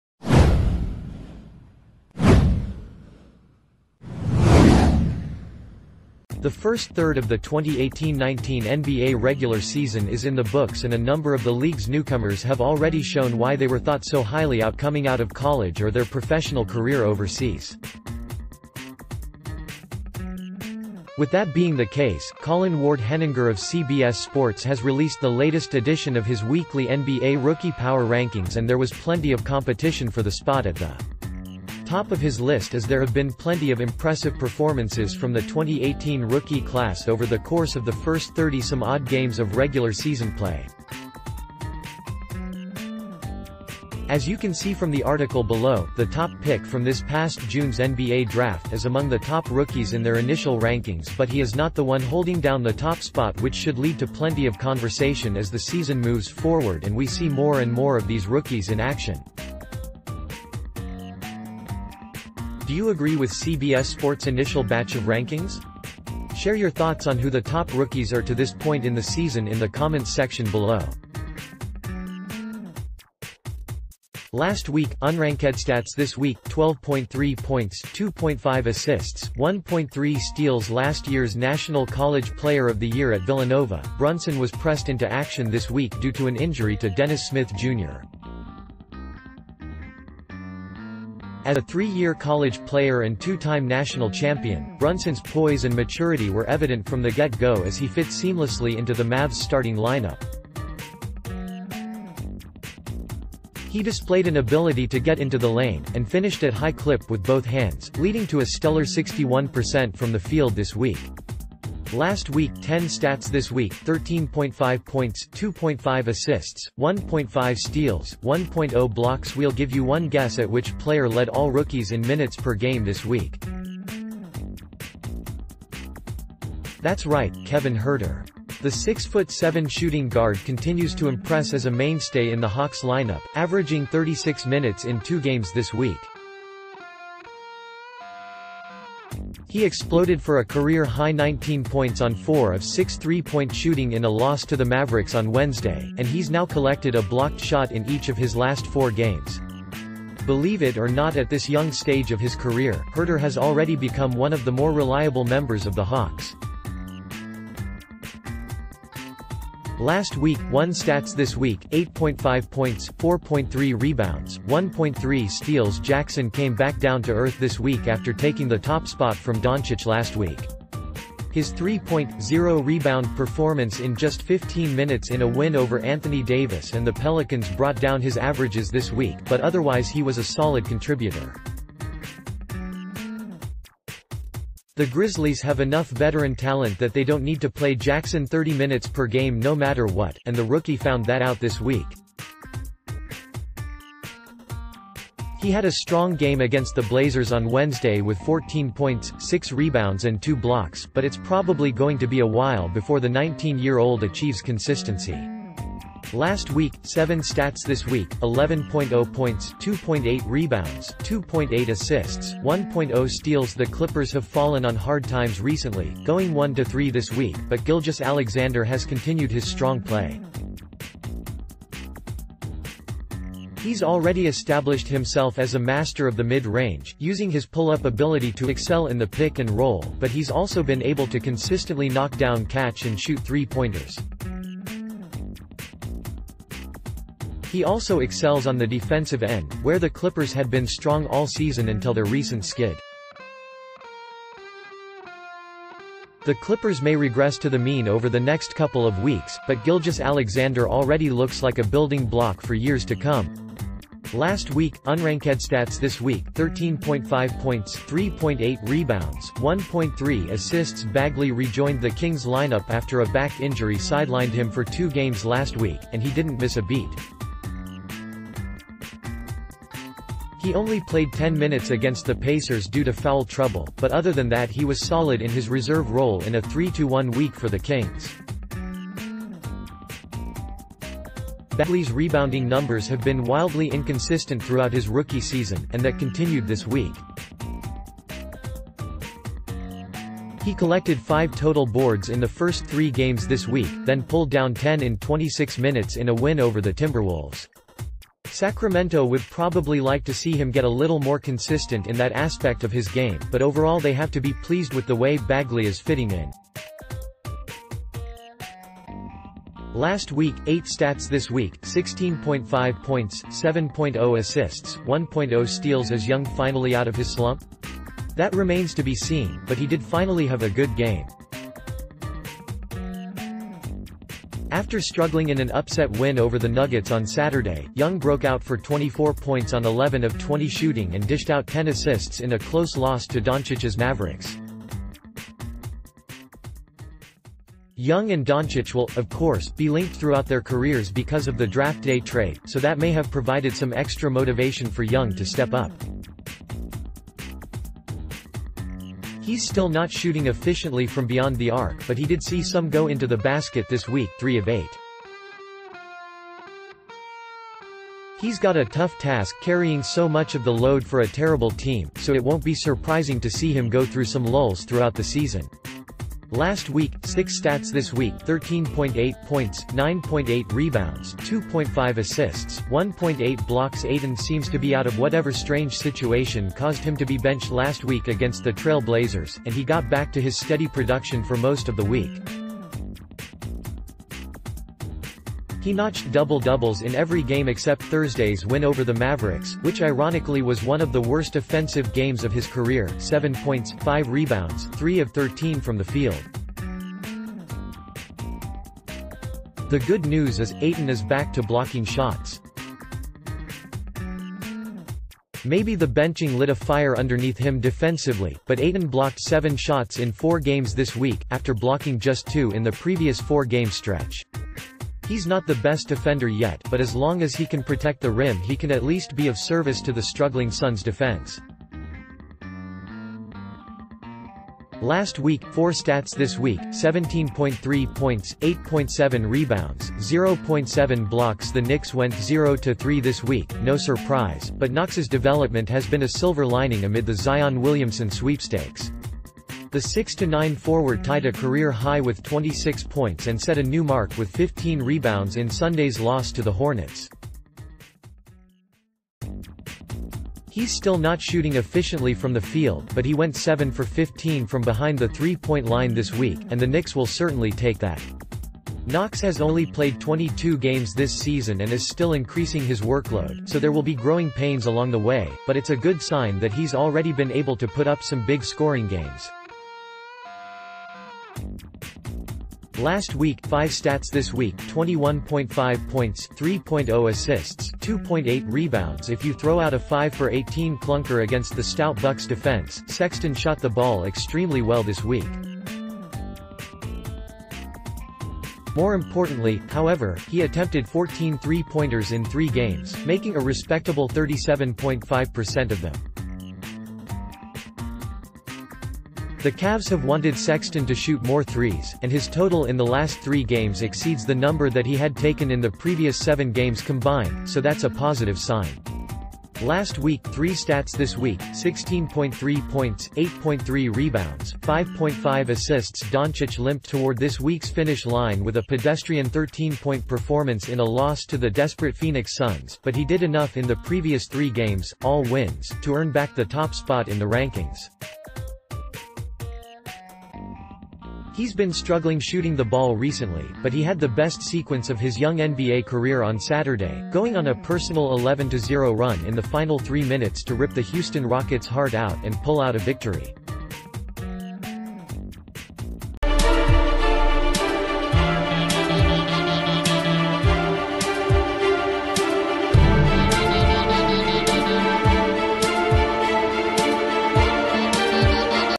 CBS Sports updates their NBA Rookie Power Rankings
The first third of the 2018-19 NBA regular season is in the books and a number of the league's newcomers have already shown why they were thought so highly out coming out of college or their professional career overseas. With that being the case, Colin Ward-Henninger of CBS Sports has released the latest edition of his weekly NBA Rookie Power Rankings and there was plenty of competition for the spot at the top of his list as there have been plenty of impressive performances from the 2018 rookie ...